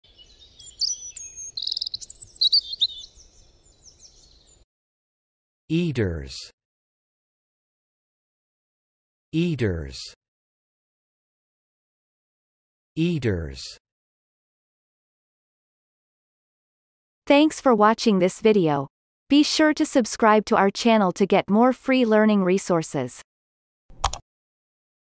How to pronounce eaters - Vocab Today
The pronunciation of the word eaters in Amercian accent is demonstrated in this video. If the word is from another language, such as brand name, it will be pronounced in its original language. Based on the word frequency, this is of 3 stars difficulty.